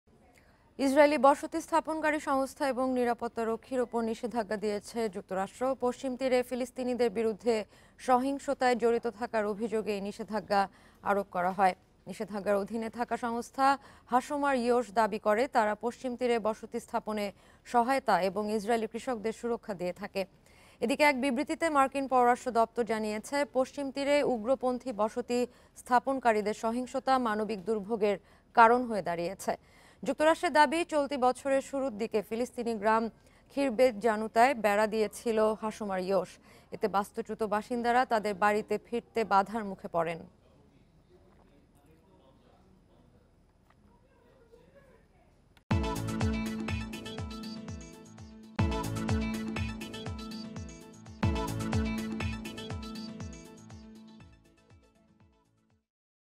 ইসরায়েলে যে নিষেধাজ্ঞা দিল যুক্তরাষ্ট্র | Israel | US | World
ইসরায়েলি বসতি স্থাপনাকারী সংস্থা ও নিরাপত্তারক্ষীর ওপর নিষেধাজ্ঞা দিয়েছে যুক্তরাষ্ট্র। পশ্চিম তীরে ফিলিস্তিনিদের বিরুদ্ধে সহিংসতায় জড়িত থাকার এই নিষেধাজ্ঞা আরোপ করা হয়।
The US has imposed sanctions on Israeli settlement agencies and security forces. The ban was imposed on engaging in violence against Palestinians in the West Bank.